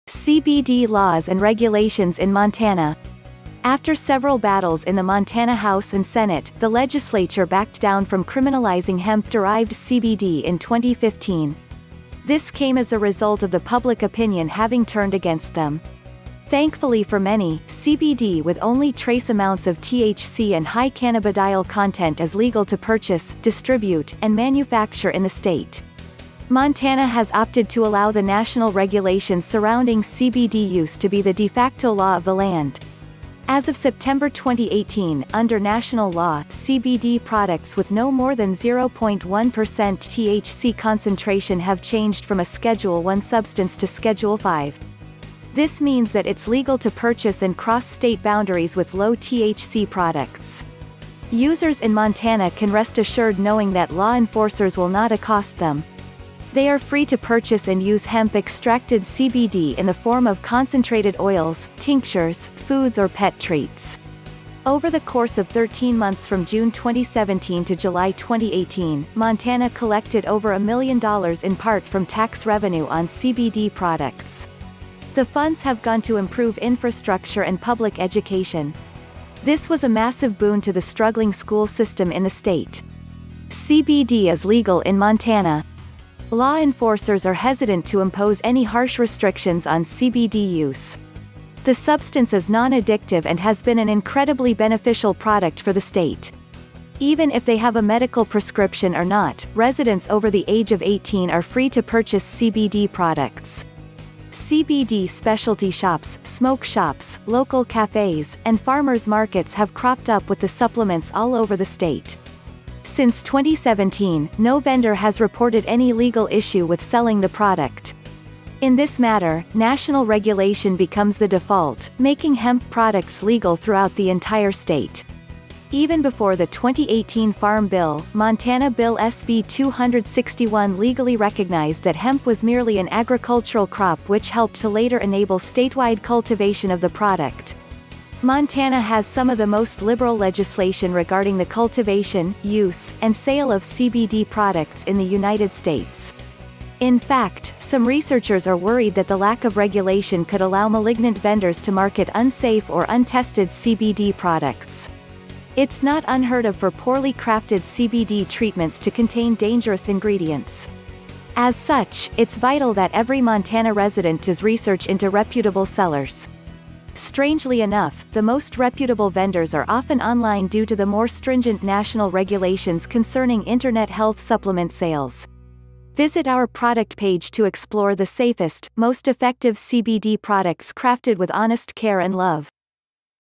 Is CBD legal in Montana?
The state of Montana has always had a contentious history with natural health products and non-addictive substances. After the 2014 Farm Act declassified hemp as a controlled substance and instead dubbed it an agricultural product, Montana has gone back and forth on its regulations regarding CBD for consumer use. So, what is the current status, is CBD legal in Montana?